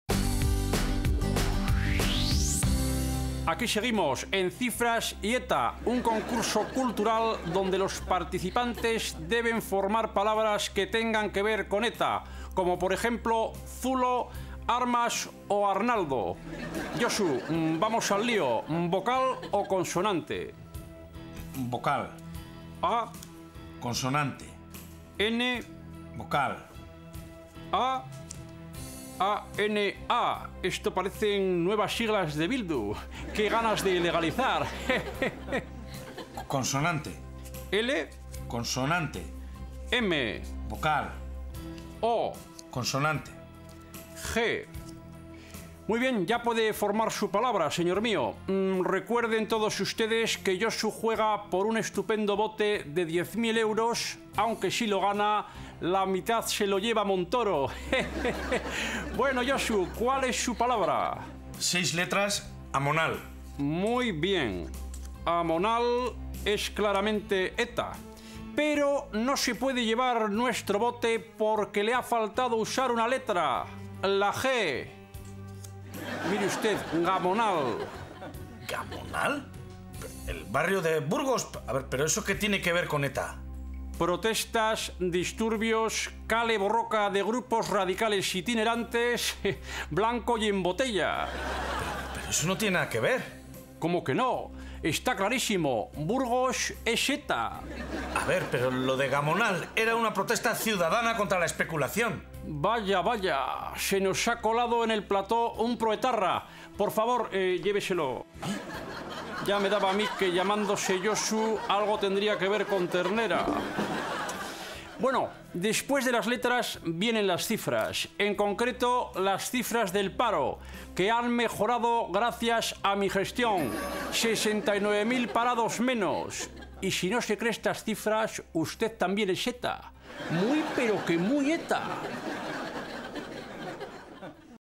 Rajoy lo tiene claro, Gamonal es ETA
Mariano Rajoy se ha puesto a presentar "Cifras y ETA". Durante el concurso, ha dejado claro que Gamonal también es ETA.